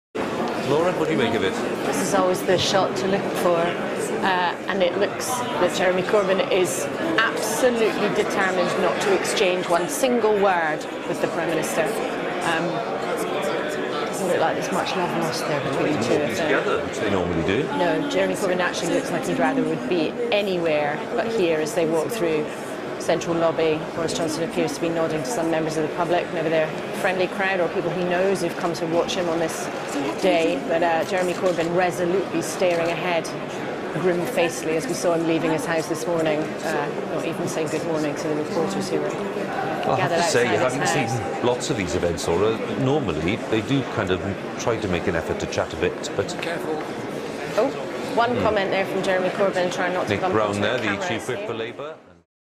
Jeremy Corbyn and Boris Johnson walk in silence to Queen's speech
MPs processed from the House Commons to the House of Lords after being summoned by Black Rod. Boris Johnson and Jeremy Corbyn walked side by side, but seemed to be pointedly avoiding sharing any words.